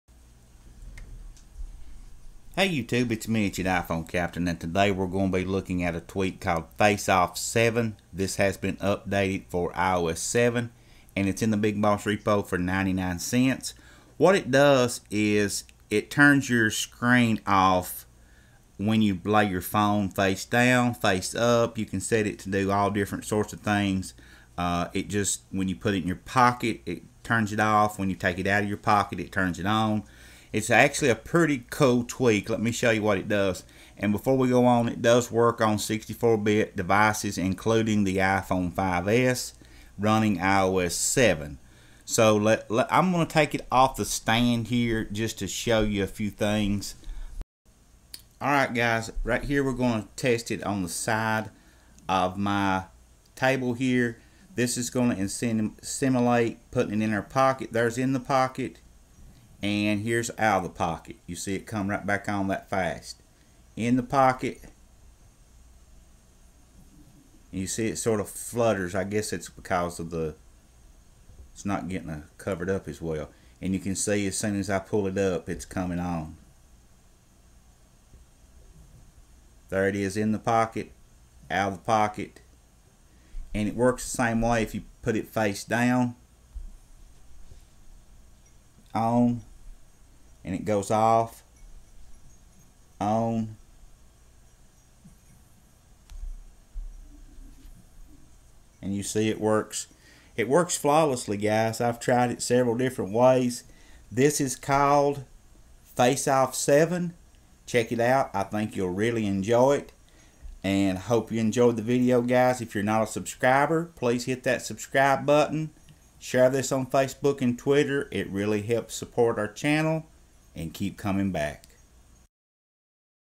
iOS 7 Jailbreak 2014: FaceOff 7 Turn Screen Off Automatically
FaceOff 7 turns screen off automatically using proximity sensor.

SOCIAL US